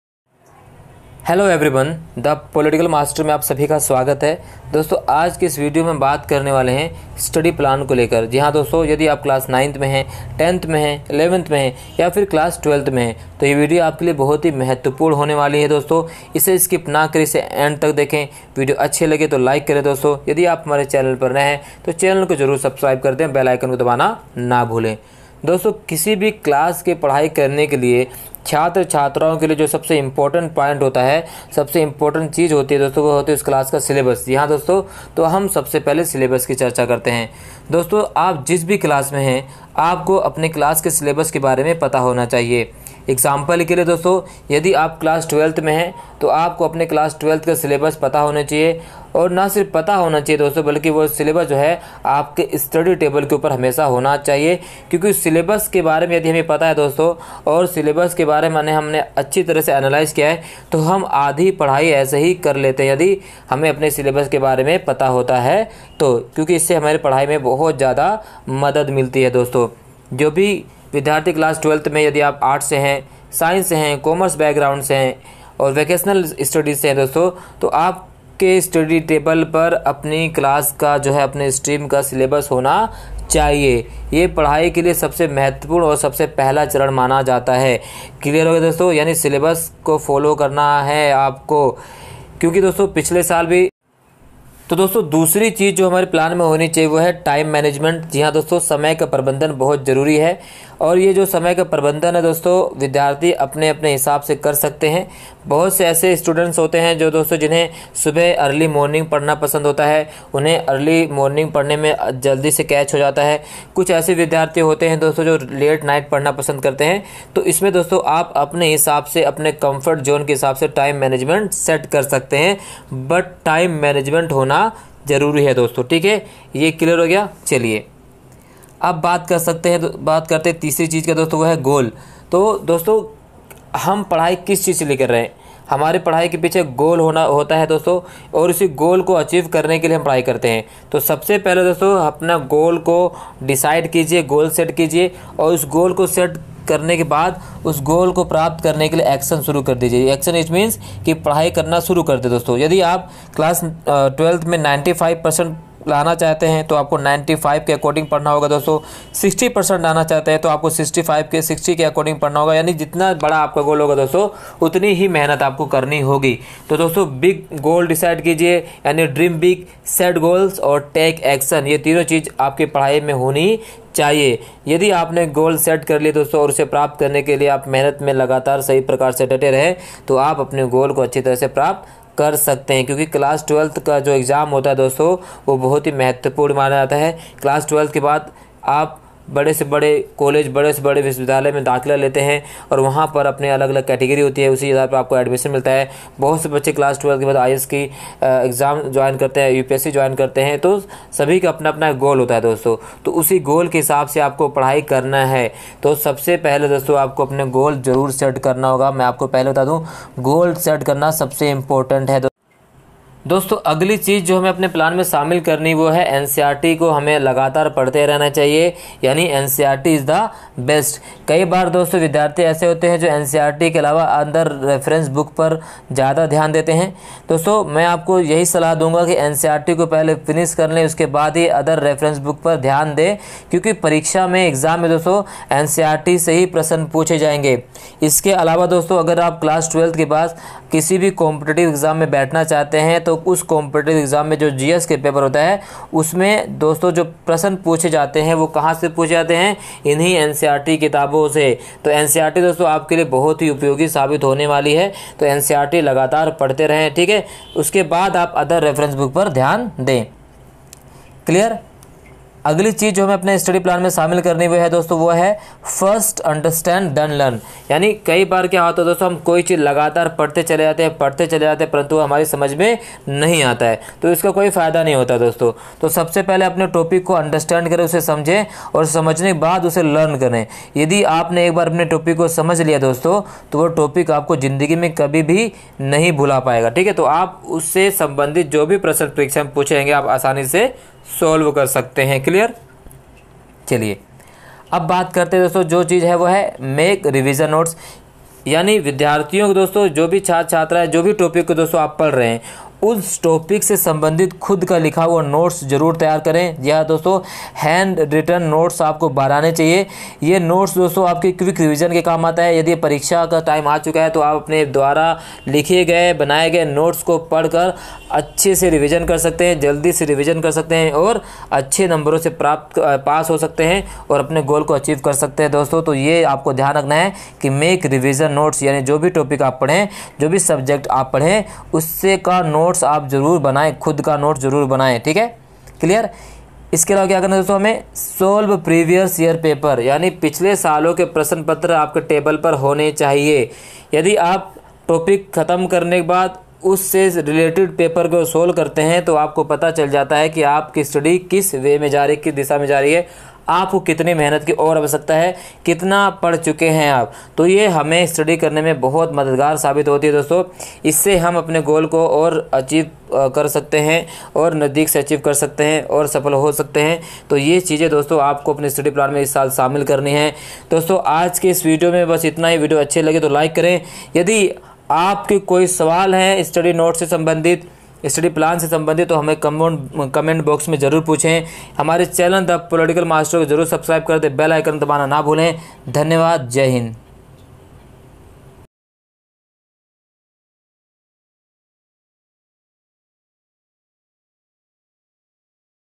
How to set your study plan | न्यू सेशन 2021-22 में 90% लाने के लिए पढ़ाई कैसे शुरू करें
इस वीडियो में हम बात करने वाले हैं कि क्लास नाइंथ टेंथ इलेवंथ और ट्वेल्थ में पढ़ाई कैसे करें

2021 22 ke liye padhaai kaise shuru kre

new session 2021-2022 ke liye study plan kaise banaye